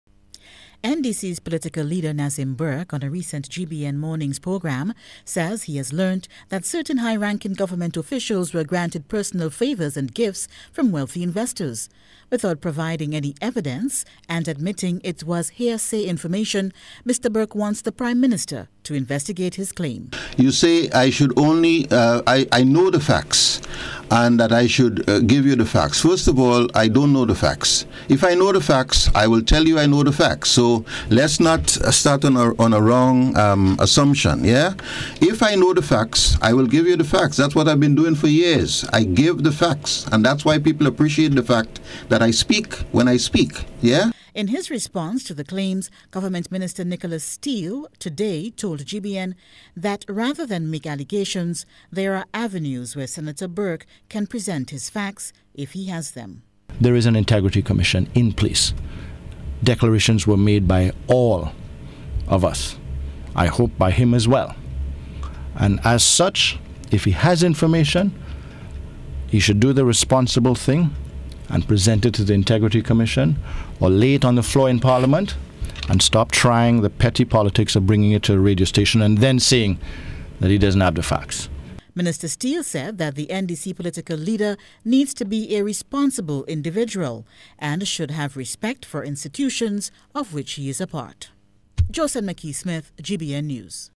Allegations Of Corruption 06.01.16
A former government minister wants the government to investigate, what he calls, “issues pointing to corruption among top government ministers and officials".
However Minister Of Health, Nicholas Steele, has responded to the matter, in an exclusive interview with GBN today.